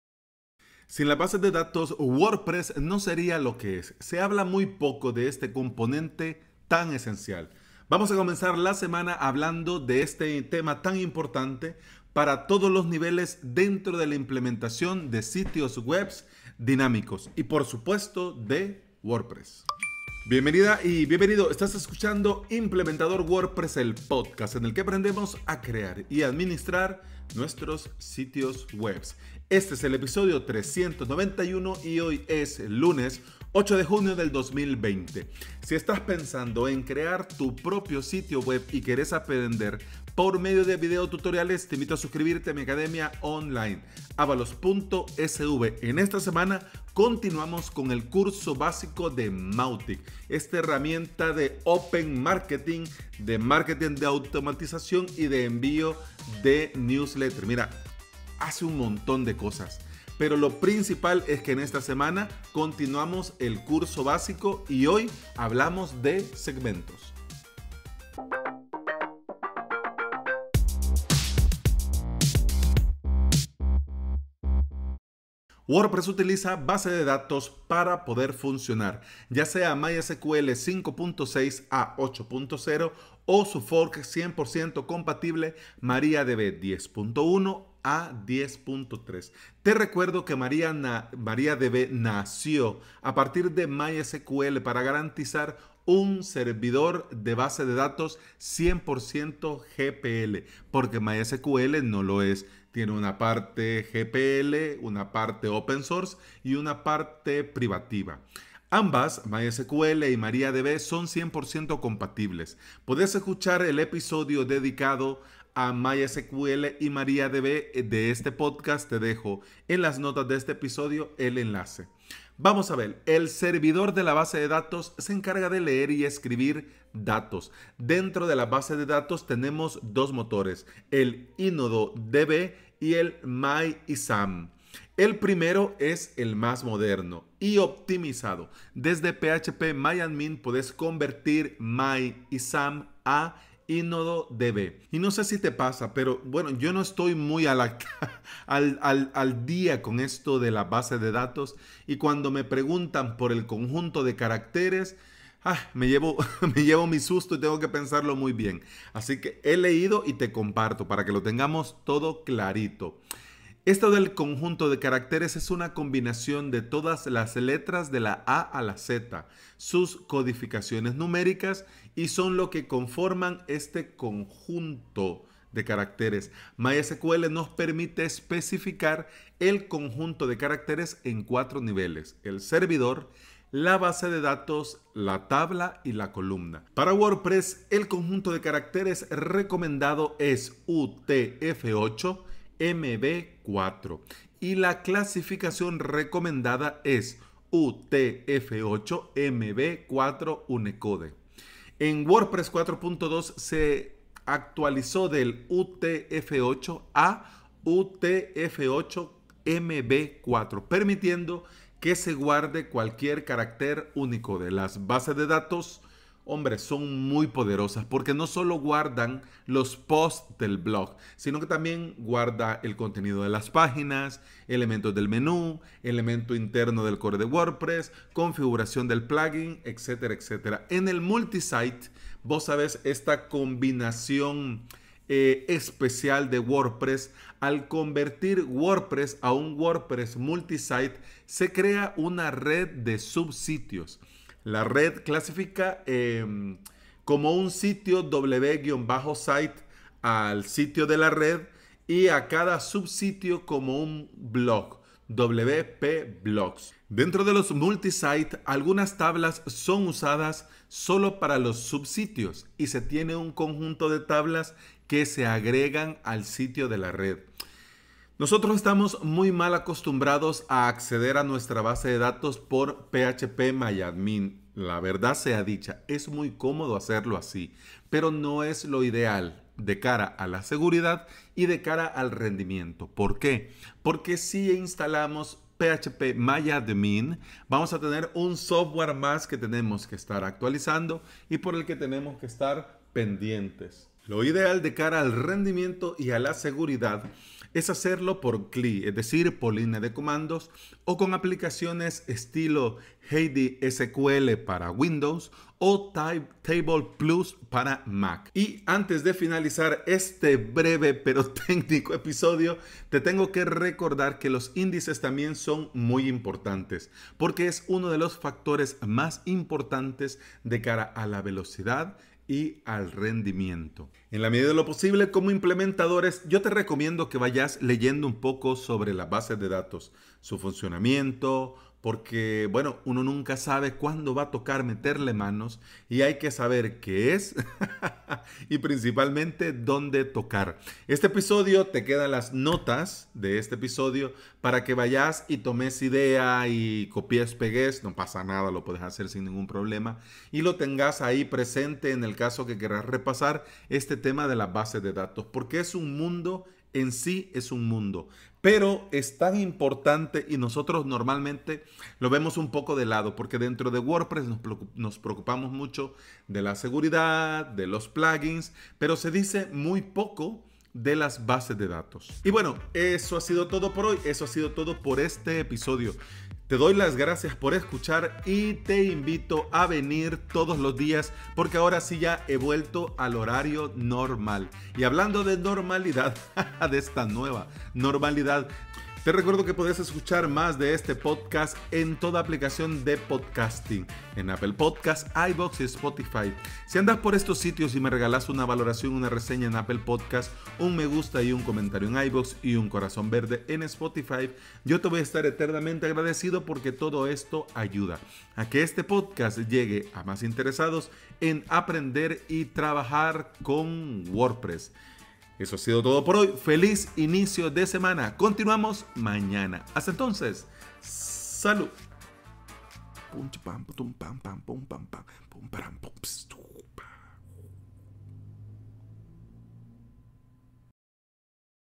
Las base de datos y WordPress
Sin las bases de datos WordPress no seria lo que es, se habla muy poco de este componente tan esencial… Comenzamos la semana hablando de este tema importante para todos los niveles dentro de la implementación de sitios webs dinámicos.

• WP utiliza base de datos MySQL 5.6 a 8.0
• O su fork 100% compatible MariaDB 10.1 a 10.3
• Te recuerdo que MariaDB nació a partir de MySQL para garantizar un servidor de DB 100% GPL
• Son 100% compatibles
• Podés escuchar el episodio dedicado: MySQL y MariaDB
• El servidor de la base de datos
• Se encarga de leer y escribir datos
• Dentro de las bases de datos tenemos dos motores:• InnoDB
• MyISAM

• El primero el mas moderno
• Y optimizado
• Desde phpMyAdmin podés convertir de MyISAM a InnoDB
• Nos llevaremos algún susto cuando nos pregunten por el Conjunto de Caracteres…• La combinación de todas las letras de la A a la Z
• Sus codificaciones numéricas
• Son lo que forman un conjunto de caracteres

• MySQL nos permite especificar el conjunto de caracteres en cuatro niveles:• El servidor
• La BD
• La tabla
• La columna

• Para WordPress el conjunto de caracteres recomendado es utf8mb4
• Y la clasificación recomendada es utf8mb4 unicode ci
• En WP 4.2 se actual de utf8 a utf8mb4
• Permitiendo que se guarde cualquier carácter Unicode
• Las DB son poderosas
• No solo guarda los post del blog• Páginas
• Elementos de menú
• Configuración de los plugins

• El multisite…
• Al convertir WP a WP Multisite
• Se crea una red de sub sitios
• La red clasifica la red como un sitio “wp site”
• Y cada sub sitio como un blog “wp blogs”
• Algunas tablas con usadas solo por un sub sitios
• Y se tiene un conjunto de tablas que se agrega al sitio de red
• Estamos muy mal acostumbrados a acceder a nuestras DB por phpMyAdmin
• Lo ideal de cara a la seguridad y rendimiento es hacerlo por CLI o con app estilo HeidiSQL o TablePlus
• Ya llegando al final del episodio, te tengo que mencionar uno de los factores mas importantes en la velocidad y el rendimiento: Los Índices
• Y podríamos extendernos… Pero ya hemos terminado por hoy…

En la medida de lo posible, como implementador… Te recomiendo que vayas leyendo un poco sobre las DB y su funcionamiento, porque uno nunca sabe cuando tocará meterle mano y hay que saber que es y donde tocar jejeje.